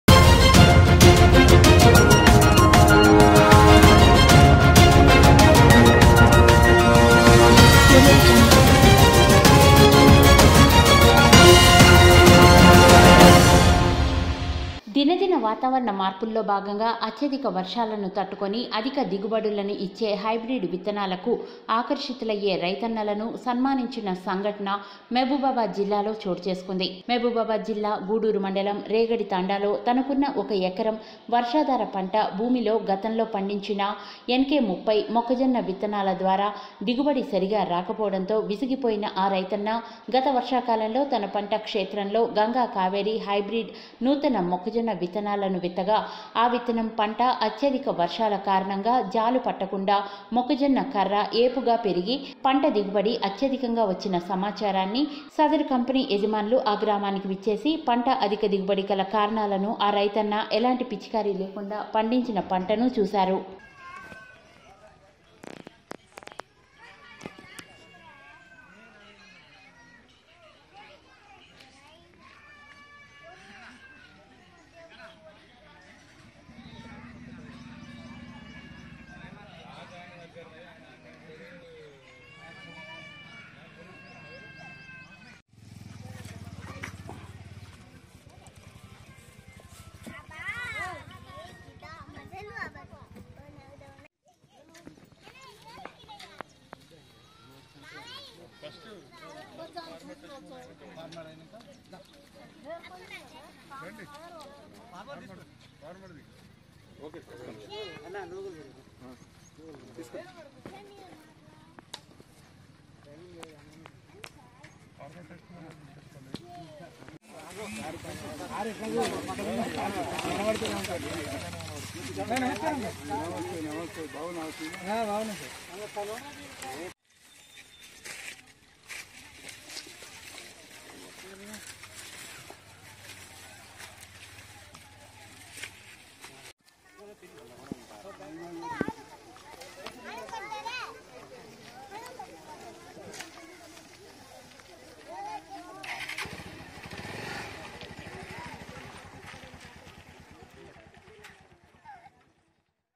దినదిన వాతావరణ మార్పులలో భాగంగా అత్యధిక వర్షాలను తట్టుకొని అధిక దిగుబడులను ఇచ్చే హైబ్రీడ్
దినదిన వాతావరణ మార్పులలో భాగంగా అత్యధిక వర్షాలను తట్టుకొని అధిక దిగుబడులను ఇచ్చే హైబ్రీడ్ విత్తనాలకు ఆకర్షితులయ్యే రైతన్నలను సన్మానించిన సంఘటన మహబూబాబాద్ జిల్లాలో చోటుచేసుకొంది
వాయిస్ ఓవర్ :మహబూబాబాద్ జిల్లా గూడూరు మండలం రేగడి తండాలో తనకున్న ఒక ఎకరం వర్షధార పంట భూమిలో గతంలో పండించిన nk30మొక్కజొన్న విత్తనాల ద్వారా దిగుబడి సరిగా రాకపోవటంతో విసిగిపోయిన ఆ రైతన్న గత వర్షాకాలంలో తన పంట క్షేత్రంలో గంగా కావేరి హైబ్రీడ్ నూతన మొక్కజొన్న విత్తనాలను విత్తగా ఆ విత్తనం పంట అత్యధిక వర్షాల కారణంగా జాలు పట్టకుండా మొక్కజొన్న కర్ర ఎపుగా పెరిగి పంట దిగుబడి అత్యధికంగా వచ్చిన సమాచారాన్ని సదరు కంపెనీ యజమానులు ఆ గ్రామానికి విచ్చేసి పంట అధిక దిగుబడి గల కారణాలు ఆ రైతన్న ఎలాంటి పిచికారి లేకుండానే పండించిన విధానం క్లుప్తంగా తెలుసుకొని రైతన్నను అభినందిస్తూ  రైతు యొక్క పంట క్షేత్రంలోనే రైతన్నను ఘనంగా షాలువాలతో సన్మానించి ఈ అతి తక్కువ పెట్టుబడితో మొక్కజొన్న పంట ఏ విధంగా అధిక దిగుబడితో పంట పండించాలో పరిసర గ్రామాల రైతన్నలకు ఆ రైతన్నతో వివరింపజేశారు